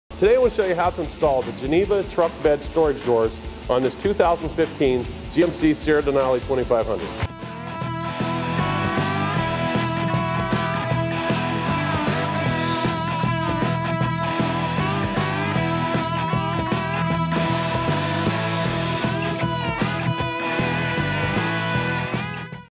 Geneva Truck Bed Storage Drawers
The Geneva Truck Bed Storage Drawers are available in bright aluminum diamond plate, black diamond plate, all steel powder-coated black or white. Choose from universal, driver, or passenger fit. Locking door to keep all your cargo safe and secure. Made in the USA.

Periscope: Follow us @RealTruck